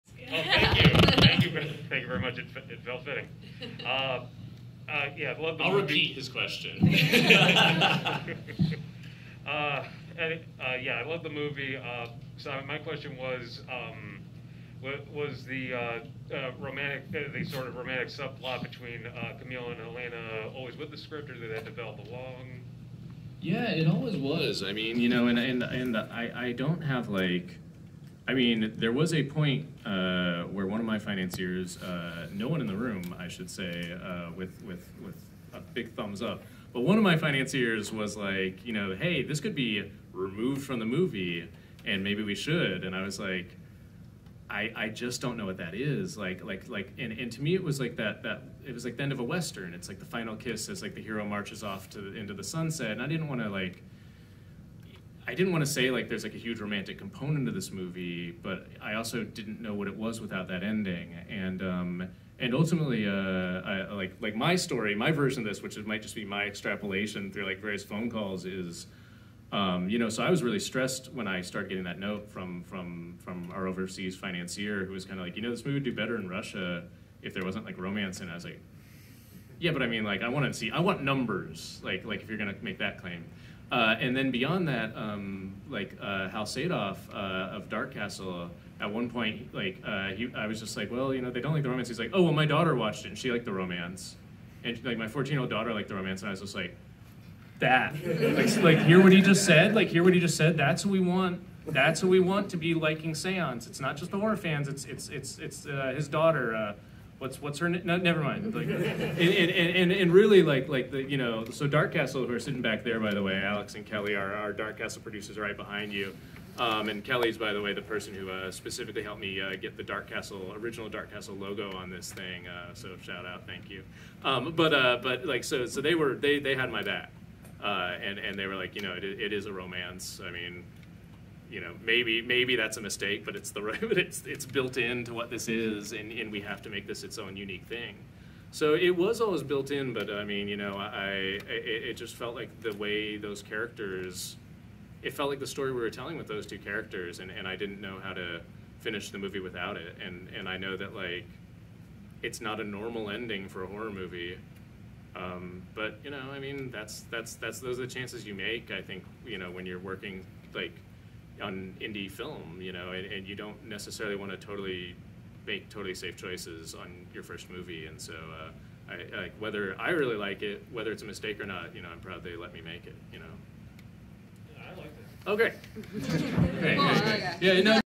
Seance Q&A With Director Simon Barrett & Actors Madisen Beaty & Inanna Sarkis: Audience Q3
On the night of May 22, 2021, Seance Director Simon Barrett and Actors Madisen Beaty & Inanna Sarkis attended Lumiere Cinema's post screening panel for a half hour long Q&A. Moderated By filmmaker (and Lumiere Cinema's Co-Owner) Peter Ambrosio,  this is just one of the questions the audience asked.